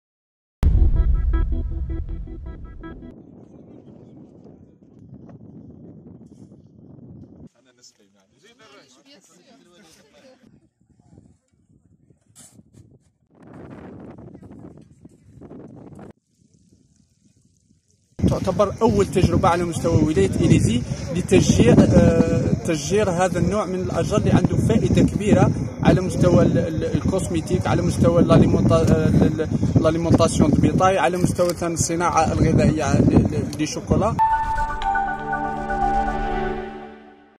إيليزي : غرس 5000 وحدة من شتلات الخروب والكاليتوس بحديقة التجارب
إيليزي (وأج)- تم غرس 5000 وحدة من شتلات الخروب و الكاليتوس بحديقة التجارب بمنطقة تين تورهة بولاية إيليزي، في إطار حملة التشجير التي أطلقتها الوزارة المنتدبة المكلفة بالبيئة الصحراوية عبر قافلة بيئية تجوب عدد من مناطق الجنوب.